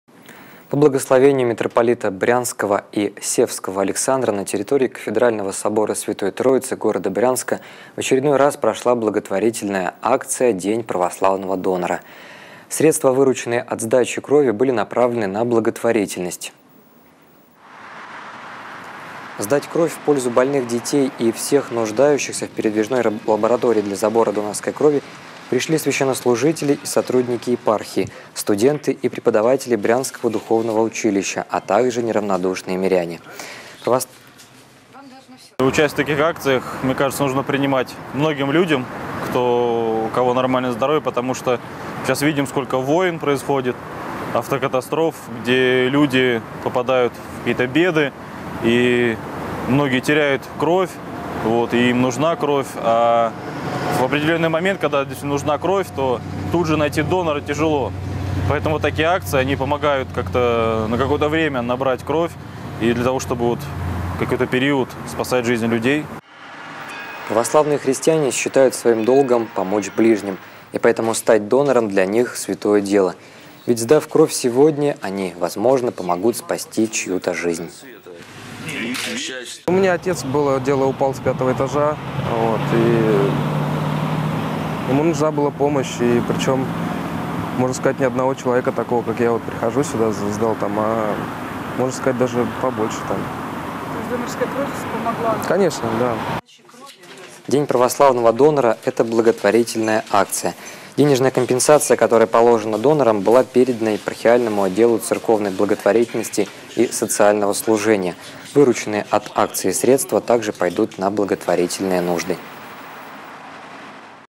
«День православного донора» в Брянске
По благословению Митрополита Брянского и Севского Александра, на территории кафедрального собора Святой Троицы города Брянска в очередной раз прошла благотворительная акция «День православного донора». Средства, вырученные от сдачи крови, были направлены на благотворительность.